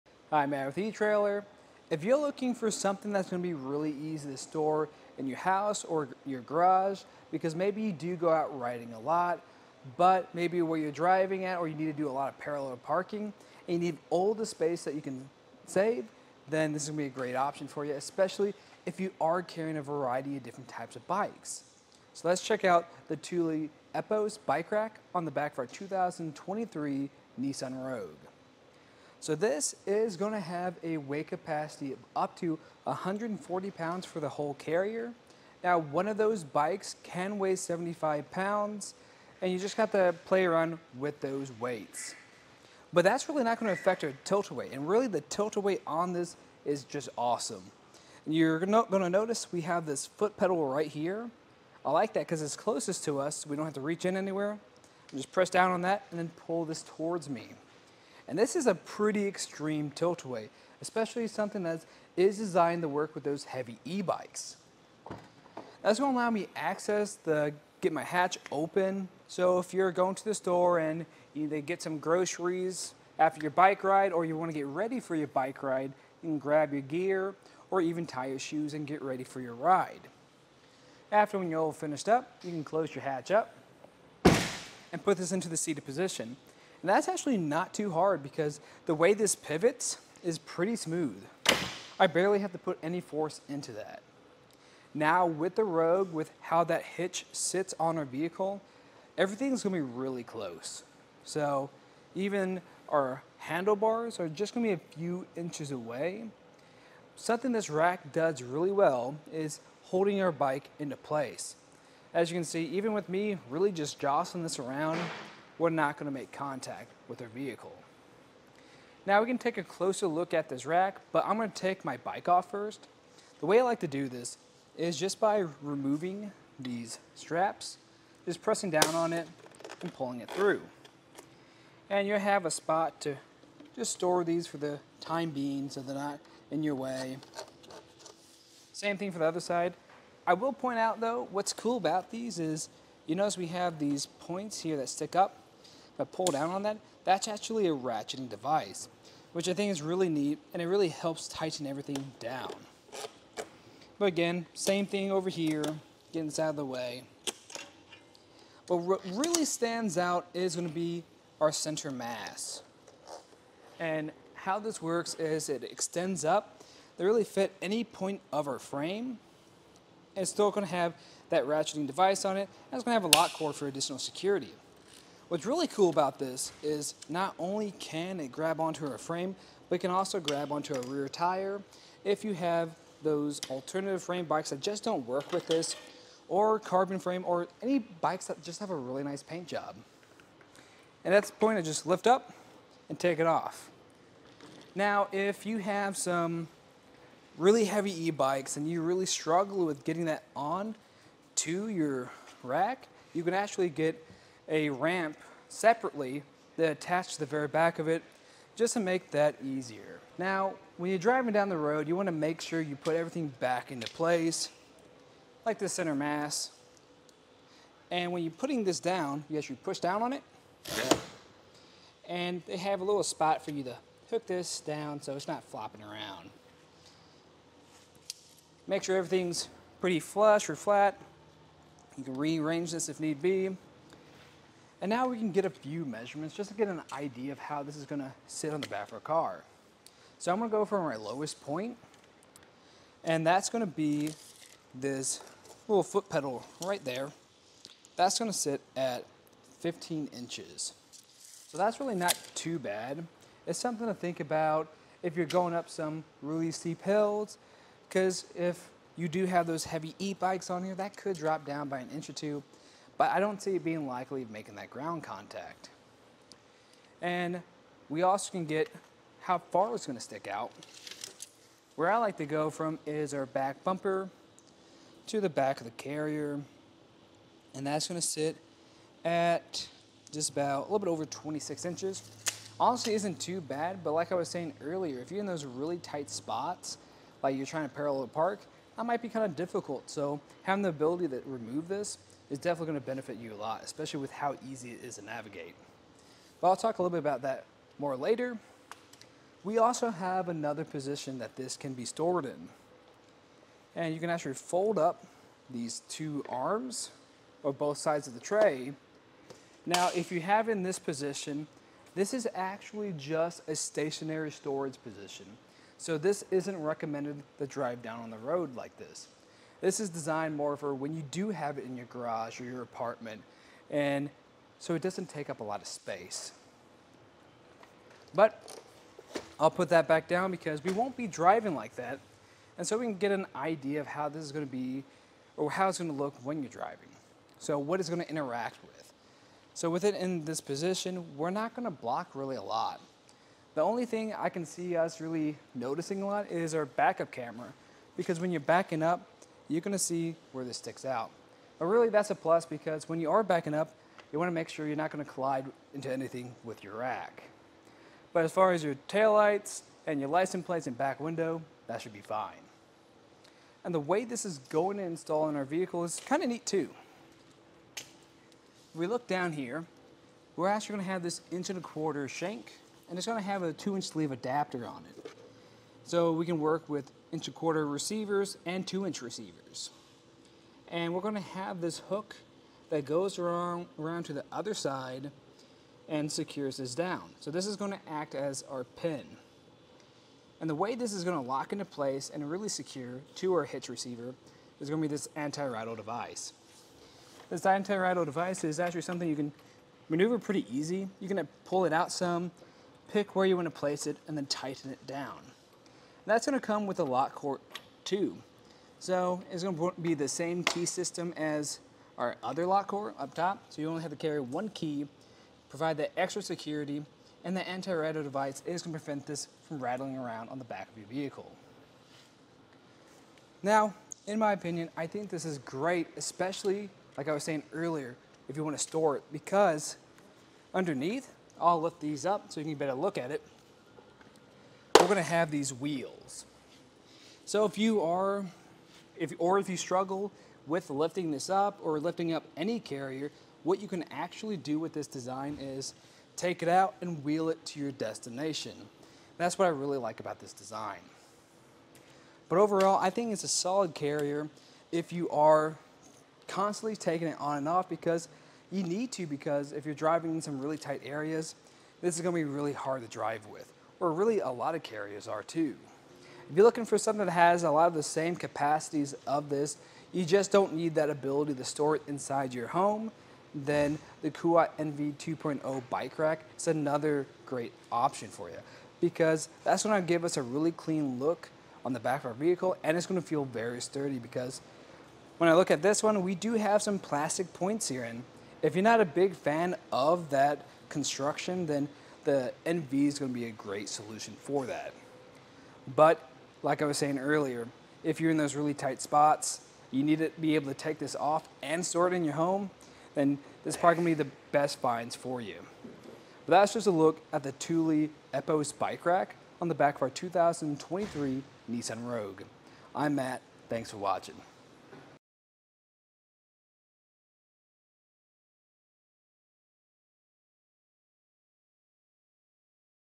etrailer | A Closer Look at the Thule Epos Bike Rack for 2 Bikes - 2023 Nissan Rogue
Hi, Matt with etrailer.  If you're looking for something that's gonna be really easy to store in your house or your garage, because maybe you do go out riding a lot, but maybe where you're driving at you need to do a lot of parallel parking and you need all the space that you can save, then this is gonna be a great option for you, especially if you are carrying a variety of different types of bikes.  So let's check out the Thule Epos Bike Rack on the back of our 2023 Nissan Rogue.  So this is gonna have a weight capacity of up to 140 pounds for the whole carrier.  Now, one of those bikes can weigh 75 pounds, and you just got to play run with those weights.  But that's really not gonna affect your tilt away, and really, the tilt away on this is just awesome.

And you're gonna notice, we have this foot pedal right here.  I like that, 'cause it's closest to us, we don't have to reach in anywhere, and just press down on that, and then pull this towards me.  And this is a pretty extreme tilt away, especially something that is designed to work with those heavy e-bikes.  That's gonna allow me to access to get my hatch open.  So if you're going to the store and get some groceries after your bike ride or you wanna get ready for your bike ride, you can grab your gear or even tie your shoes and get ready for your ride.

After when y'all finished up, you can close your hatch up, and put this into the seated position.  And that's actually not too hard, because the way this pivots is pretty smooth.  I barely have to put any force into that.  Now, with the Rogue, with how that hitch sits on our vehicle, everything's gonna be really close.  So even our handlebars are just gonna be a few inches away.

Something this rack does really well is holding our bike into place.  As you can see, even with me really just jostling this around, we're not gonna make contact with our vehicle.  Now, we can take a closer look at this rack, but I'm gonna take my bike off first.  The way I like to do this is just by removing these straps, just pressing down on it, and pulling it through.  And you'll have a spot to just store these for the time being, so they're not in your way.

Same thing for the other side.  I will point out though, what's cool about these is, you'll notice we have these points here that stick up, if I pull down on that, that's actually a ratcheting device, which I think is really neat, and it really helps tighten everything down.  But again, same thing over here, getting this out of the way.  But what really stands out is gonna be our center mast.  And how this works is it extends up to really fit any point of our frame.  It's still gonna have that ratcheting device on it, and it's gonna have a lock core for additional security.  What's really cool about this is not only can it grab onto our frame, but it can also grab onto our rear tire if you have those alternative frame bikes that just don't work with this, or carbon frame or any bikes that just have a really nice paint job.  And at this point, I just lift up and take it off.  Now, if you have some really heavy e-bikes and you really struggle with getting that on to your rack, you can actually get a ramp separately that attach to the very back of it just to make that easier.  Now, when you're driving down the road, you wanna make sure you put everything back into place, like the center mast.  And when you're putting this down, you actually push down on it.  (center mast ratcheting) And they have a little spot for you to fit this down, so it's not flopping around.  Make sure everything's pretty flush or flat.  You can rearrange this if need be.  And now, we can get a few measurements, just to get an idea of how this is gonna sit on the back of our car.  So I'm gonna go from my lowest point, and that's gonna be this little foot pedal right there, that's gonna sit at 15 inches.  So that's really not too bad.  It's something to think about if you're going up some really steep hills, 'cause if you do have those heavy e-bikes on here, that could drop down by an inch or two, but I don't see it being likely making that ground contact.  And we also can get how far it's gonna stick out.  Where I like to go from is our back bumper to the back of the carrier, and that's gonna sit at just about a little bit over 26 inches.  Honestly, isn't too bad, but like I was saying earlier, if you're in those really tight spots, like you're try